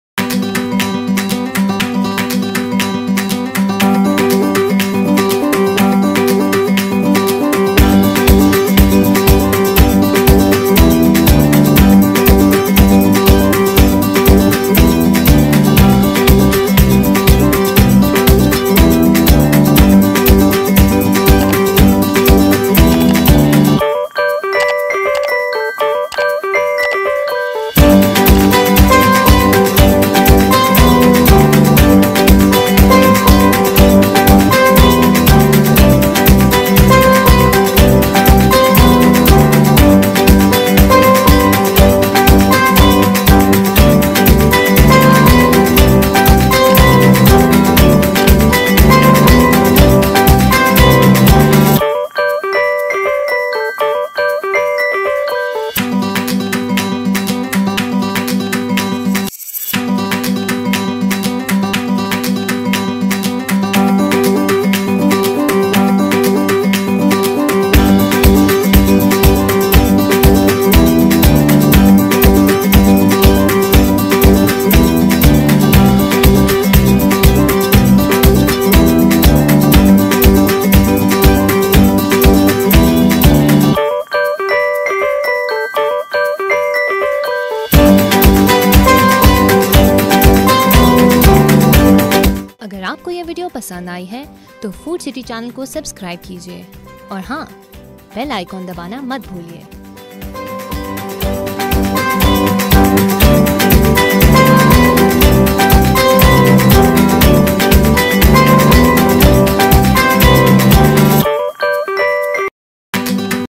Turmeric Herbal Tea | Weight loss | Best Herbal Tea Recipe by Food City
A steaming hot cup of turmeric tea is both invigorating and comforting. A beautiful blend of sweet and aromatic spices, this type of tea is both delicious and healthy for you.

Ingredients:
Water 4 cups
Turmeric Powder (Haldi Powder) 1/4 tsp
Cloves (Laung) 2 pcs
Cinnamon Stick (Darchini) 1 pcs
Ginger 1 inch pcs
Fennel Seeds (Saunf) 1 tsp
Mint 6 leaves
Sugar 1 & 1/2 tbsp
Lemon Slices as required